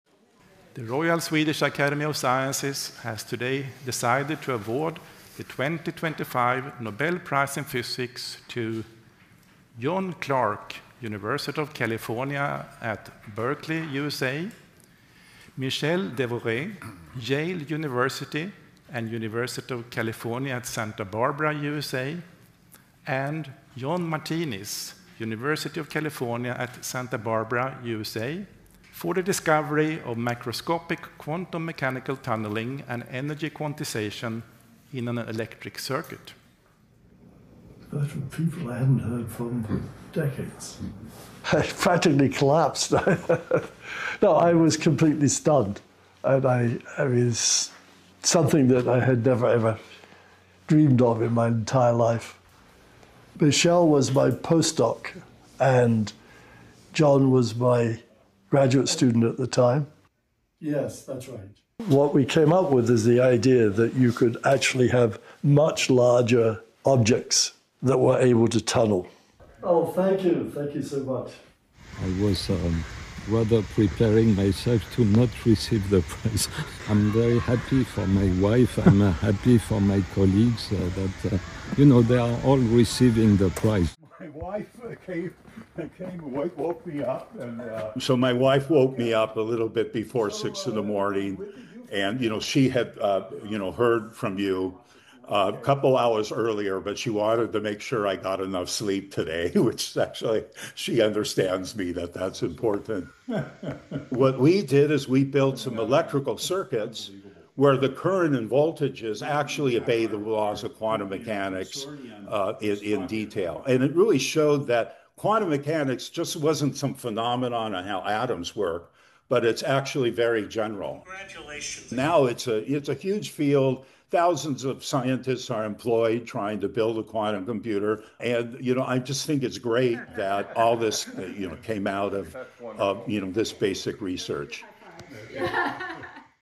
Three scientists at US universities win Nobel Prize in physics for advancing quantum technology
FOR CLEAN VERSION SEE STORY NUMBER: 4608853, 4608964, 4609001, 4608935

ASSOCIATED PRESS
Stockholm - 7 October 2025
1. Panel entering
++PARTIALLY COVERED++
2. SOUNDBITE (English) Hans Ellegren, Secretary-General, Royal Swedish Academy of Sciences:
"The Royal Swedish Academy of Sciences has today decided to award the 2025 Nobel Prize of Physics to John Clarke, University of California at Berkeley, USA, Michel, H Devoret, Yale University and University of California at Santa Barbara, USA and John Martinis, University of California at Santa Barbara USA, for the discovery of microscopic quantum mechanical tunnelling and energy quantisation in an electric circuit."
3. Various of laureates on screen

ASSOCIATED PRESS
Berkeley, California - 7 October 2025
4. Various of John Clarke reading email on laptop at his home
++PARTIALLY COVERED++
5. SOUNDBITE (English) John Clarke, 2025 Nobel Prize winner for Physics, UC Berkeley physics professor:
"I practically collapsed. No, I was completely stunned. I mean, it's something that I had never, ever dreamed of in my entire life."
6. Various of Clarke talking on phone
++PARTIALLY COVERED++
7. SOUNDBITE (English) John Clarke, 2025 Nobel Prize winner for Physics, UC Berkeley physics professor:
"Michel (Devoret) was my postdoc and John (Martinis) was my graduate student at the time. What we came up with is the idea that you could actually have much larger objects that were able to tunnel."
8. Clarke standing in dining room

ASSOCIATED PRESS
Santa Barbara, California – 7 October 2025
++PARTIALLY COVERED++
9. SOUNDBITE (English) Michel Devoret, Nobel Prize in physics winner:
"I was rather preparing myself to not receive the prize."
++WHITE FLASH++
10. SOUNDBITE (English) Michel Devoret, Nobel Prize in physics winner:
"I'm very happy for my wife, I'm happy for my colleagues that, you know, they are all receiving the prize."
11. John Martinis entering home ++NIGHT SHOTS++
++PARTIALLY COVERED++
12. SOUNDBITE (English) John Martinis, Nobel Prize in physics winner:
“So my wife woke me up a little bit before six in the morning. And she had heard from you a couple hours earlier, but she wanted to make sure I got enough sleep today, which actually — she understands me, that that's important."
13. Various of Martinis standing in living room
14. Various of Martinis looking at computer
++PARTIALLY COVERED++
15. SOUNDBITE (English) John Martinis, Nobel Prize in physics winner:
“What we did is we built some electrical circuits where the current and voltages actually obey the laws of quantum mechanics in detail. And it really showed that quantum mechanics just wasn't some phenomenon on how atoms work, but it's actually very general."
16. Martinis speaking to AP reporter on Zoom call
++PARTIALLY COVERED++
17. SOUNDBITE (English) John Martinis, Nobel Prize in physics winner:
"Now it's a huge field. Thousands of scientists are employed trying to build a quantum computer. And I just think it's great that all this came out of this basic research.”
18. Martinis posing for a photograph with his wife
STORYLINE:
John Clarke, Michel H. Devoret and John M. Martinis won the Nobel Prize in physics on Tuesday for research on the weird world of sub-atomic quantum tunneling that advances the power of everyday digital communications and computing.

One of the winners said that quantum mechanics research already has wound up in our everyday communications.

AP reporters were later able to speak with Martinis, once his wife decided it was late enough to wake him up.